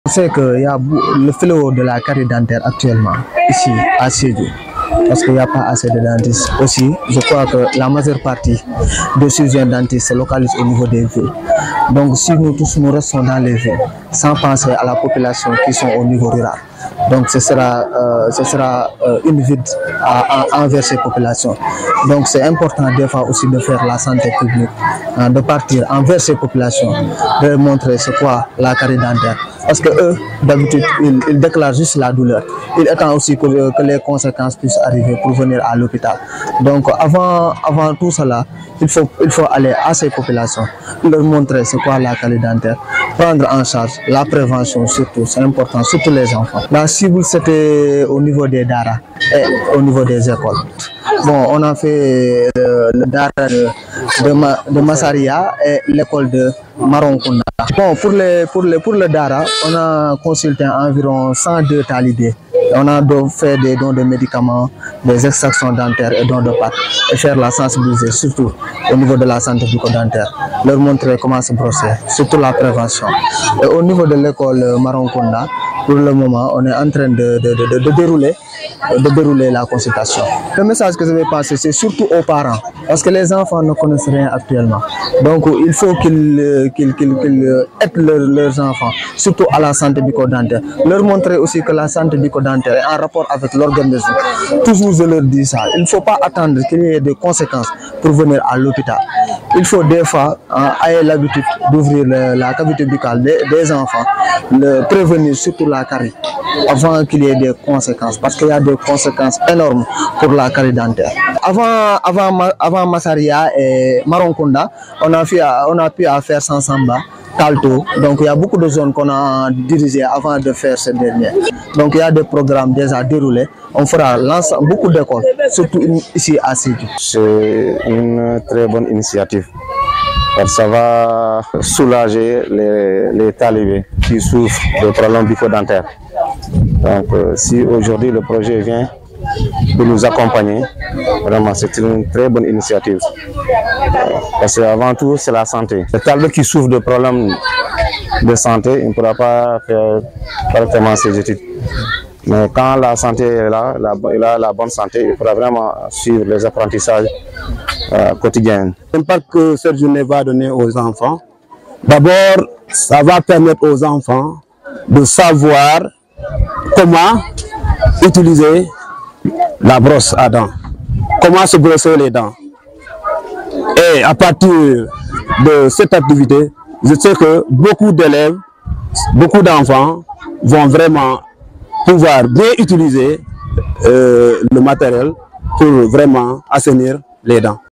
Santé bucco-dentaire plus de 102 talibés consultés à Massaria dans la commune de Bambali.
Des villages de la commune de Bambali ont accueilli une activité de santé communautaire consacrée à l'hygiène bucco-dentaire destinée essentiellement à des daras et des écoles élémentaires. C'est une initiative de l'équipe de chirurgiens dentistes du district sanitaire de Sédhiou qui ont allié dans  les villages cibles sensibilisation, traitement et distribution de pâte dentifrice et de broches.